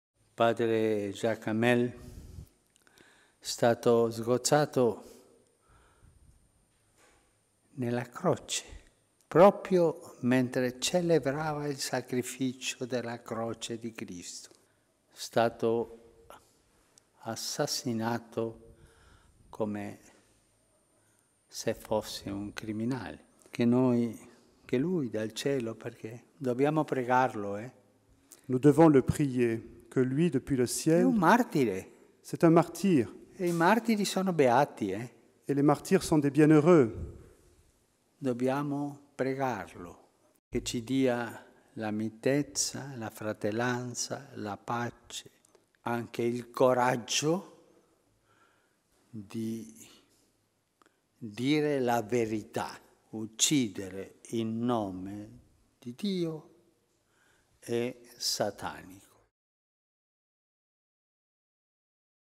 Ein seliger Märtyrer: Papst Franziskus über Pfarrer Jacques Hamel
Papst Franziskus würdigt den von Islamisten ermordeten Pfarrer Jacques Hamel als seligen Märtyrer in seiner Predigt am 14. September 2016. Video: CNA/EWTN News